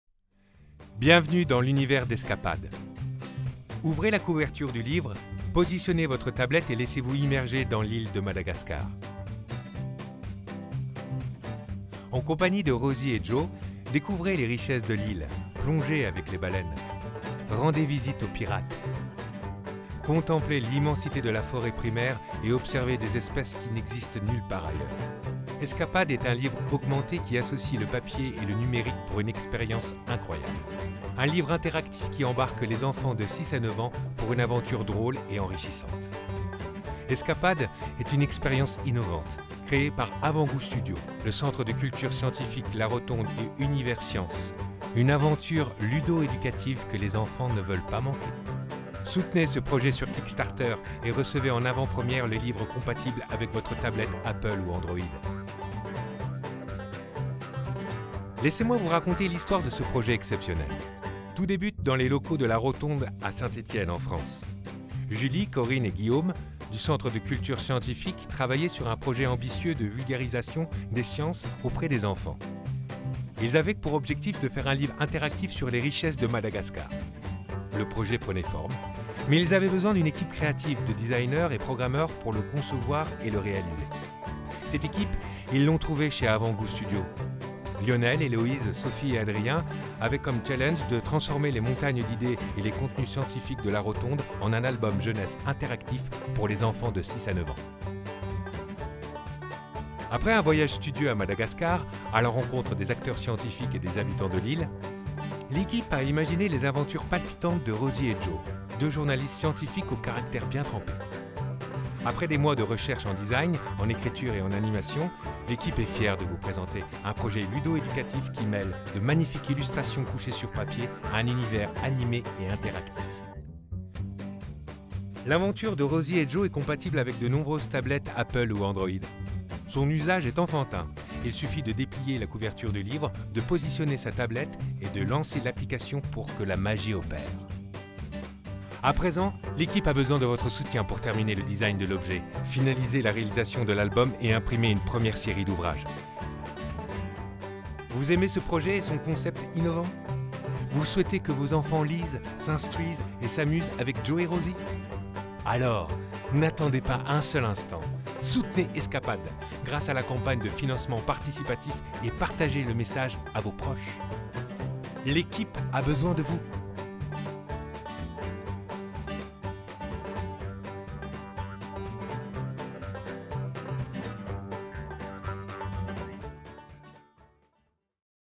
Esca'Pad à Madagascar, soutenez le projet sur Kickstarter !
Esca'Pad à Madagascar ! Une histoire scientifique et immersive, une innovation numérique pour les enfants de 6 à 9 ans.

Quand la couverture du livre se transforme en support pour tablette !
Grâce à un dispositif innovant, le carnet de voyage s'enrichit comme par magie de vidéos, animations, jeux, expériences, informations scientifiques et ambiances sonores qui plongent les enfants dans une aventure interactive à la découverte de la grande île rouge, en compagnie de Rosalie et Georges, reporters scientifiques.